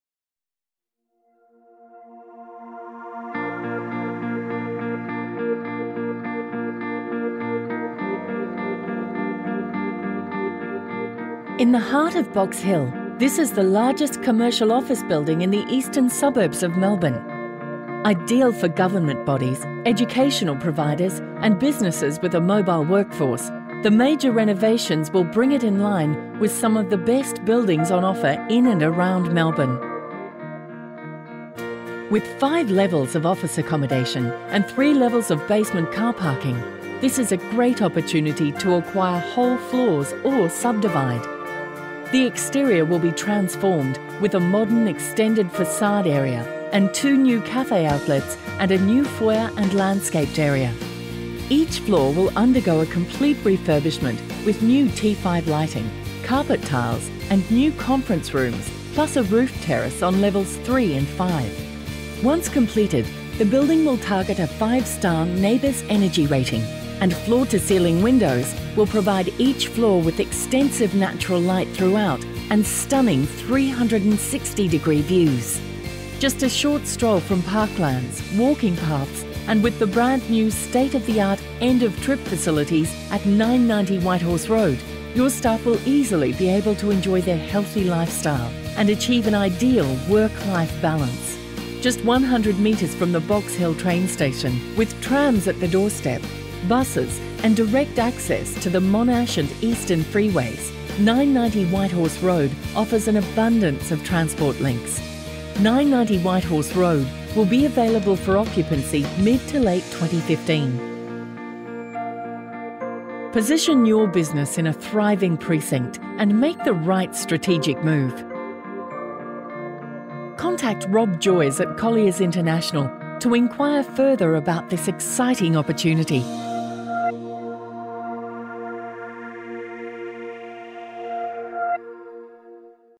990 Whitehorse Road, Box Hill
990 Whitehorse Road, Box Hill is set to become Victoria's largest available Melbourne metropolitan office space. It provides an opportunity to strategically move your business into an A-Grade premise in one of Melbourne's most thriving precincts.